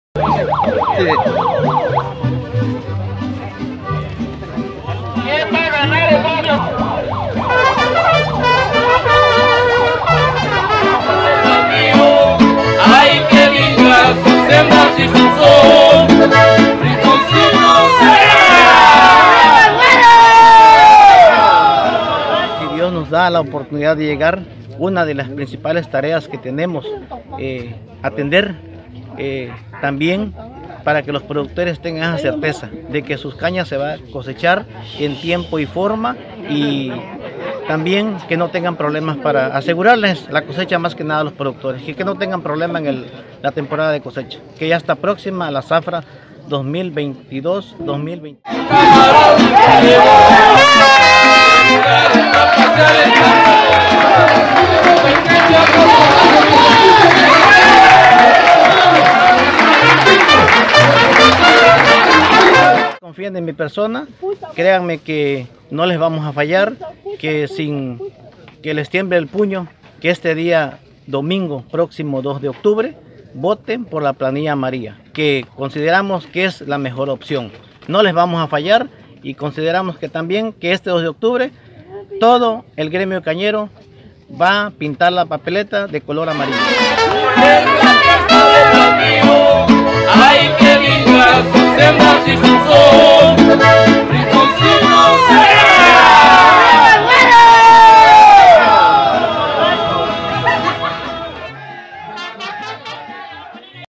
De última hora, sector cañero se une y respalda el proyecto del Güero Ocaña
• Con propuestas y trabajo de unidad cierra campaña
Huixtla Chiapas 01 de octubre de 2022.- Todo listo para la elección a la dirigencia de la Unión Local de Productores de Caña de Azúcar del Ingenio Huixtla, Gilberto Ocaña Flores, un grupo de productores acordaron por unanimidad respaldar su apoyo y votar en favor de la Planilla Amarilla del “Güero Ocaña”.
Productores campesinos del sector cañero, quienes a pocas horas se reunieron para refrendar su decidido apoyo este domingo reconociendo que es la mejor opción para las próximas moliendas toda vez que su proyecto es la mejor propuesta y porque quienes integran su planilla son de  productores con muchos años de experiencia y que no están “quemados”, pero más que nada,    se identifican con el sector cañero ya que el equipo que podrá sacar adelante a los productores de la situación en que se encuentran.
El “Güero Ocaña” dijo estar comprometido con este gremio campesino donde buscará mejorar condiciones de trabajo, producción y desarrollo de la caña de azúcar, donde los beneficios se vean reflejados en la estabilidad de las familias productoras.
Solo faltan unas horas para que este próximo domingo a primeras horas del día se realicen las votaciones y por la tarde se reflejen los resultados, donde se esperan las familias cañeras elijan al mejor proyecto.